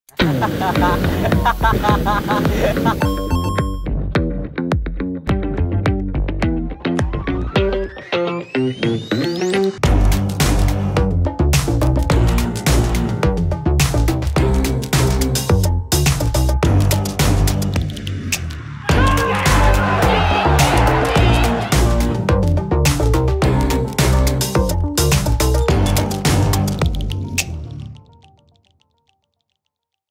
EG ESPN Commercial
Who thought giving Jiizuke a flamethrower was a good idea? We did, of course! We're setting ESPN ablaze with the LIVEEVIL spirit.